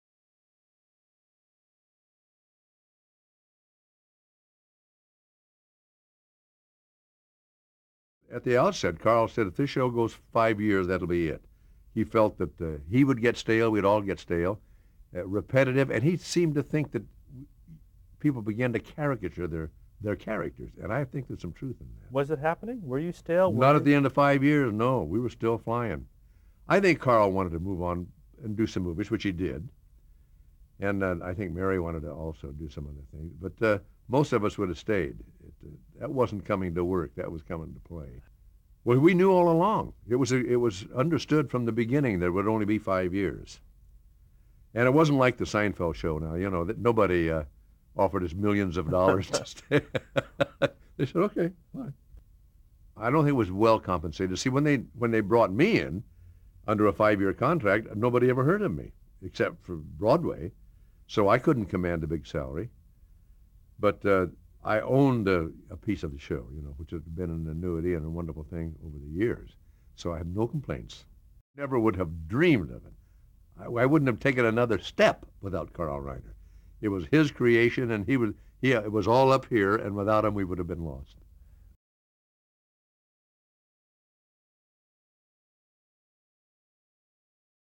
Dick Van Dyke discusses the finale of "The Dick Van Dyke Show" - EMMYTVLEGENDS.ORG
See Dick Van Dyke's full Archive of American Television interview